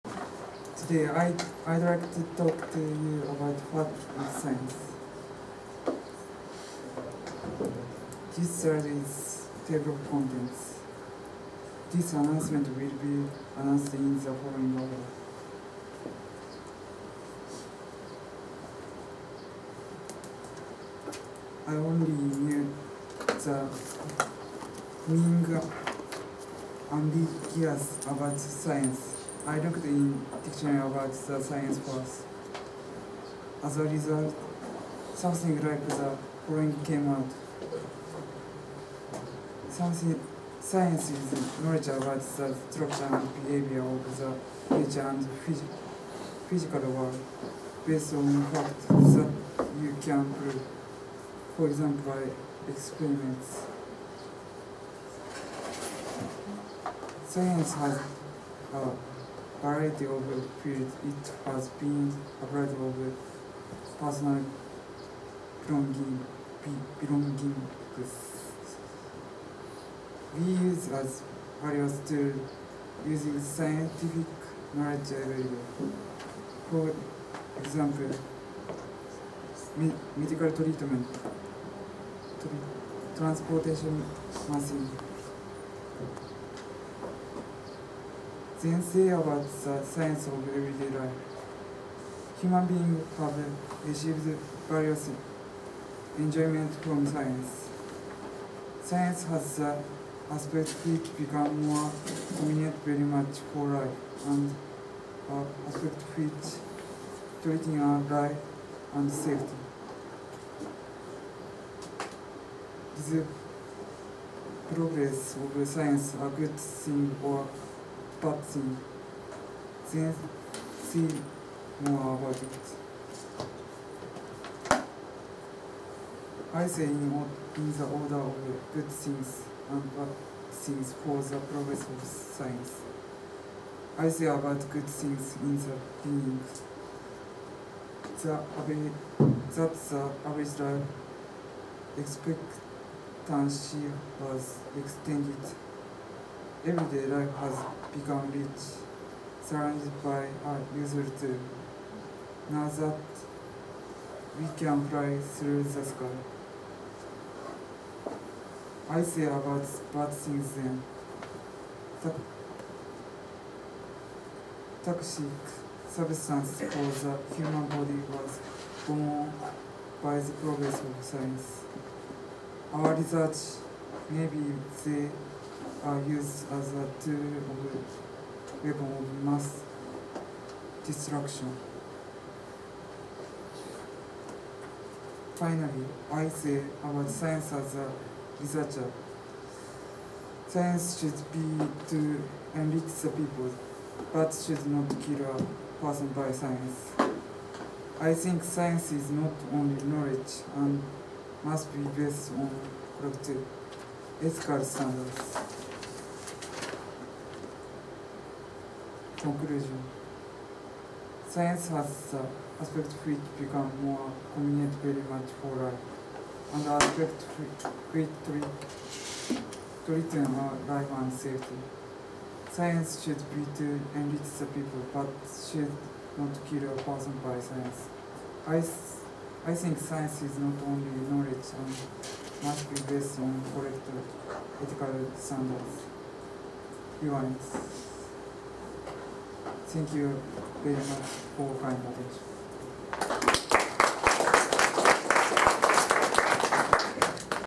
What is science?
LIFE presentation workshop -- 2012.

Engineering graduate student English course at University of Fukui, Japan.
Teacher: E. Hitzer.